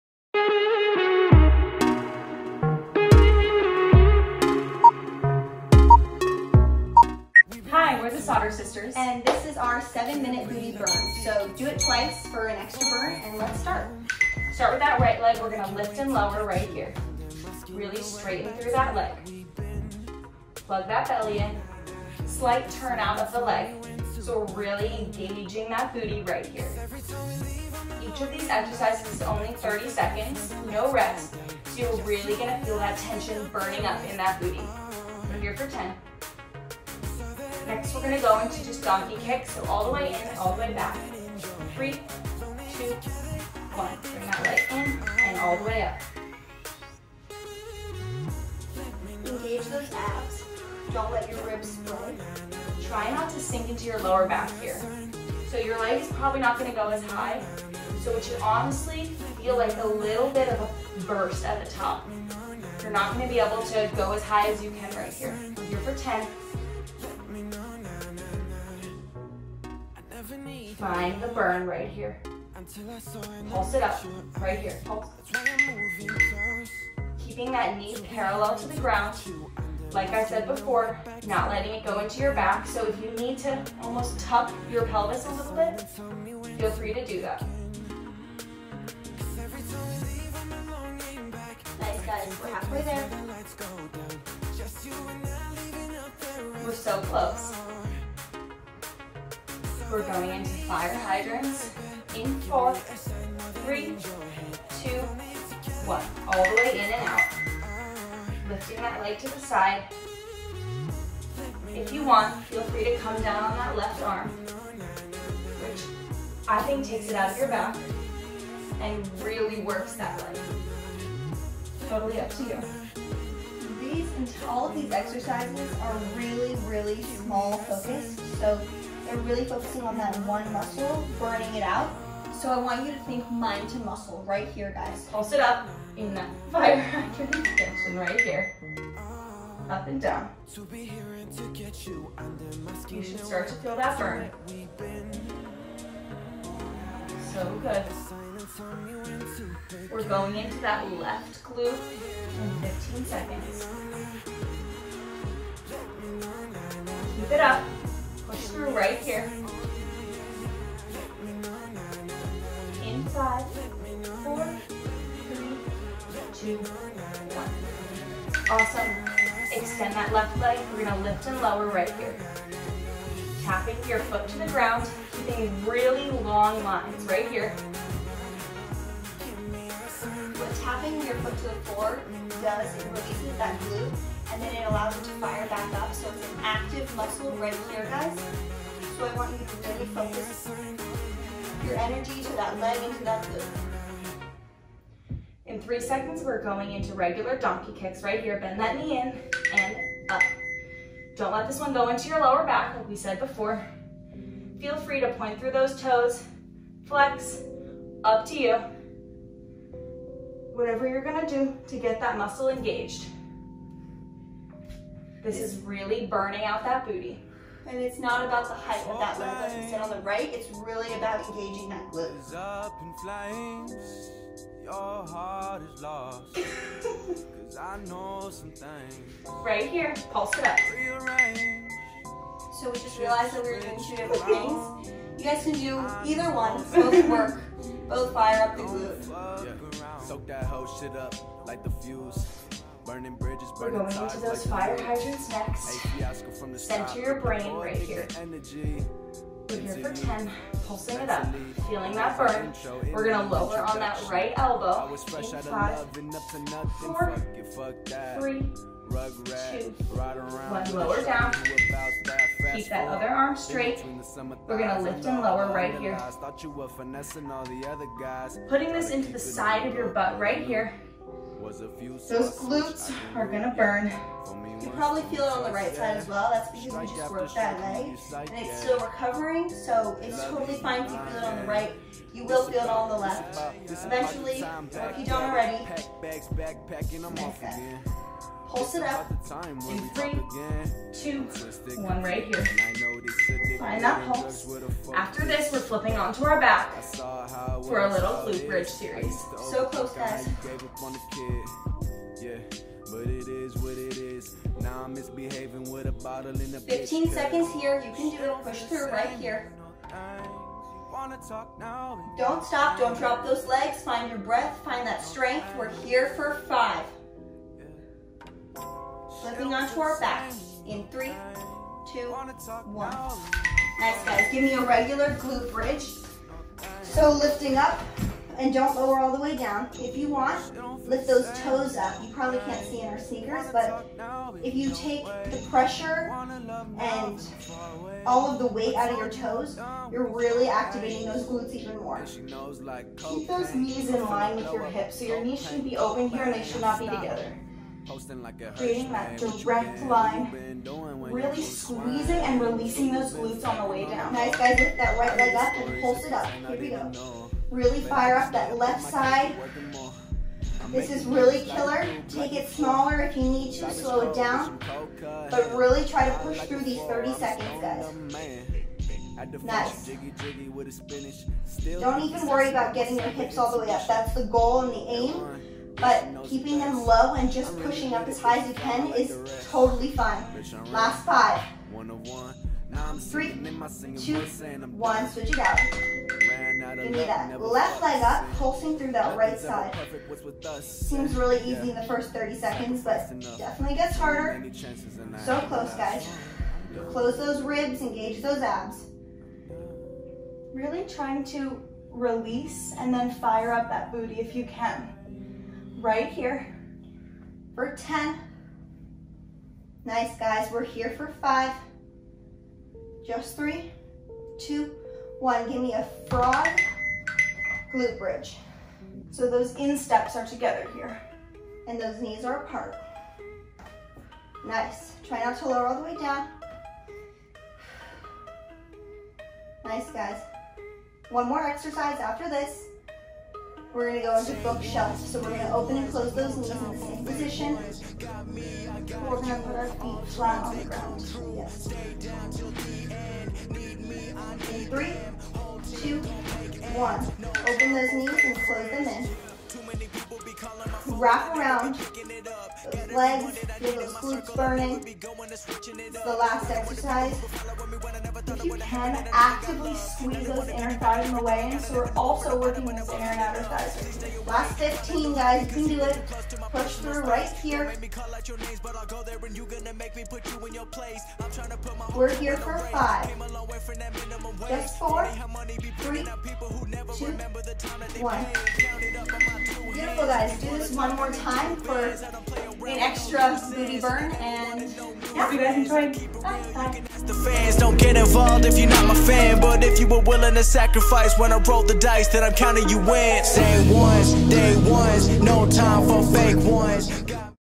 7 Minute Booty Burn x TheSautterSisters //
Hi Babes!
Expect a lot more videos like this-- workouts, what we eat, & our lifestyle. Hope you are having a great day! Add this to your workout to really sculpt that booty and feeeel the burn. Comment one thing you are looking forward to (does not matter how big or small).
For us: The social distanced picnic we are having tonight with family friends!
Go get that sweat on and post a selfie to your story so we can see that sweaty glow!
xx